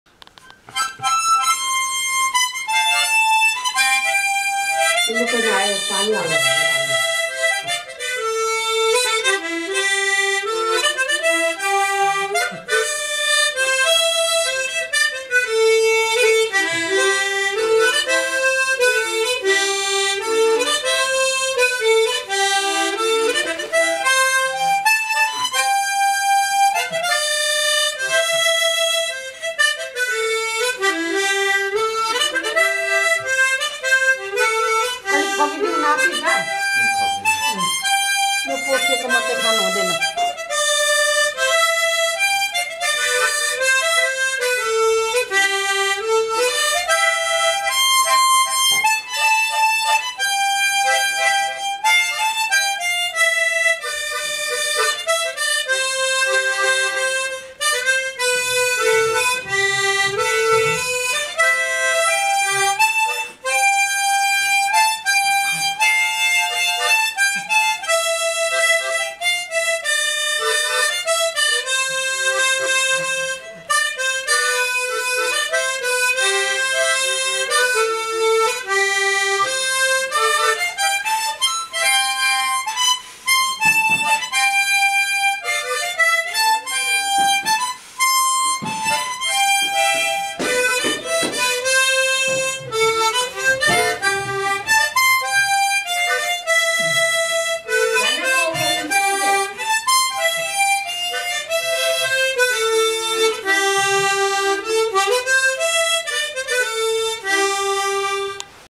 Mouth Organ (Harmonica)
Mouth Organ (Harmonica) is one of the endanger musical instruments, limited to seniors generation of Nepalese people.  Baje is playing with so much of enthusiasm, inspiring to learn more about it, on Jan 18, 2013, Friday at 9 PM at Brampur, Itahari.